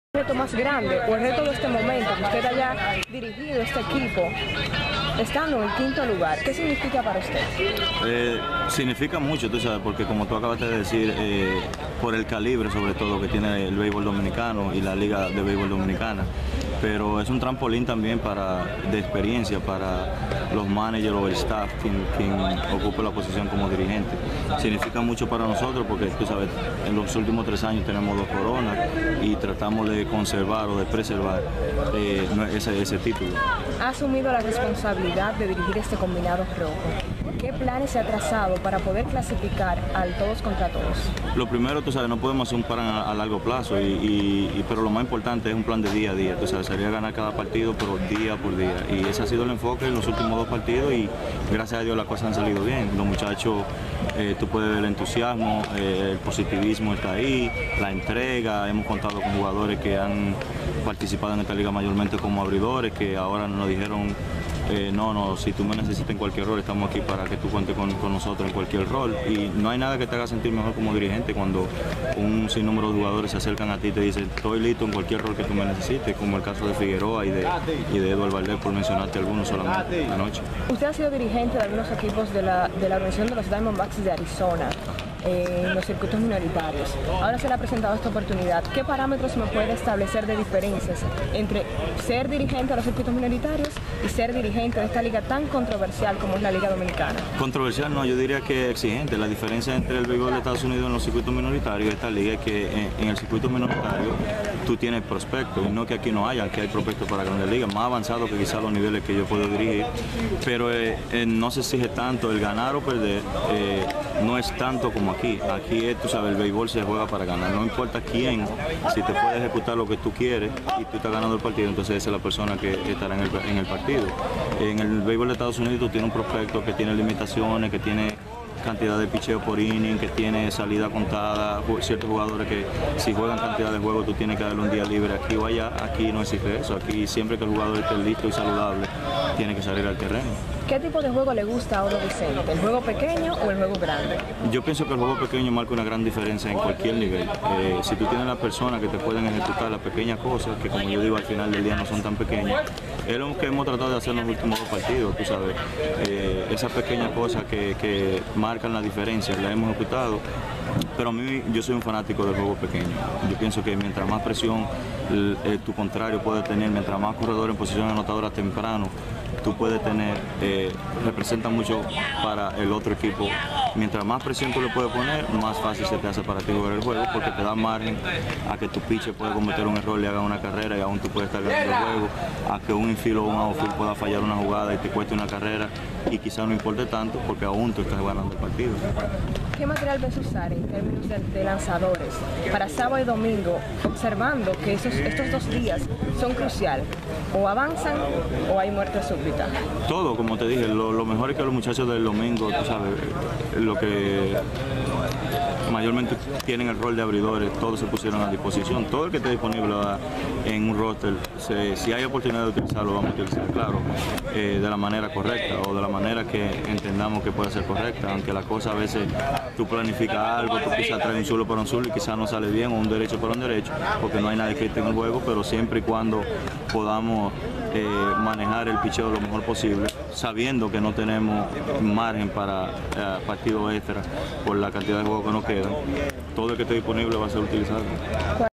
Audo Vicente cuenta con todo el mundo en el Escogido
El dirigente de los Leones del Escogido confia en sus lanzadores.